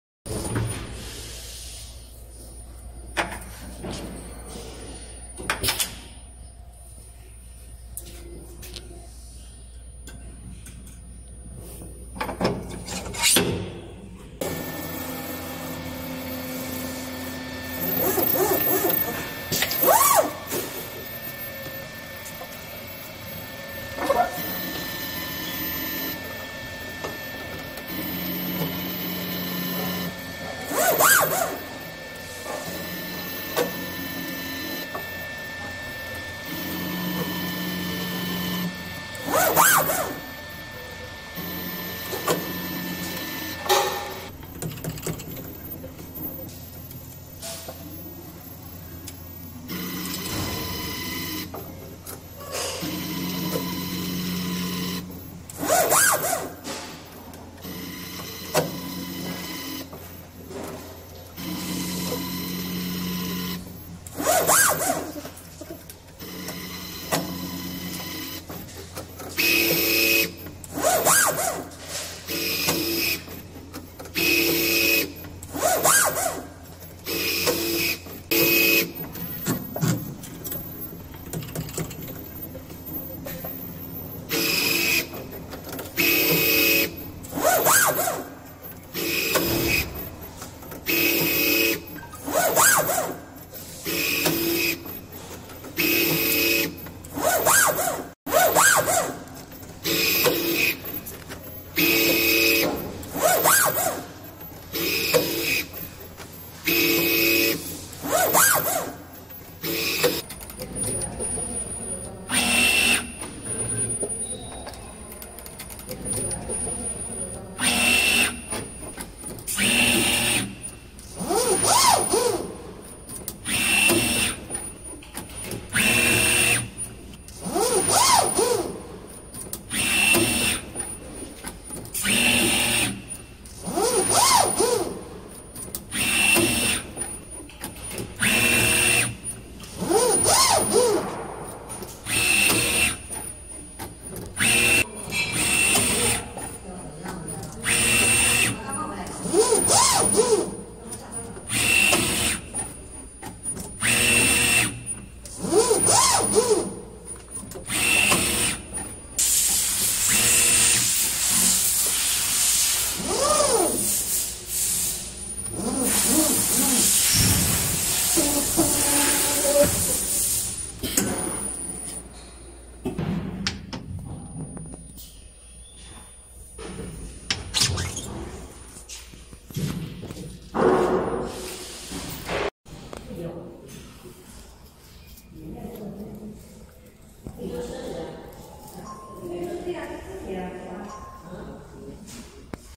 Automatic BLDC Motor Winding Machine Process | 14 strands wire Needle Winding Technology
The BLDC winding machine combines with the latest needle winding technology and is known for its speed and stability in producing.14 strands wire can be wound at the same time to improve the stator winding speed.

The automatic needle motor winding machines is high-speed stator winders and is suitable for brushless /BLDC motor stator coil winding,manually loading / unloading,the machine automatically complete mold clamping,winding,wire clamping and cutting,it can achieve multi tapping (set on HMI).The stator slot NO. and turn NO. can be set on HMI.It can do multi strands wire parallel winding, and the machine has remote diagnosis function.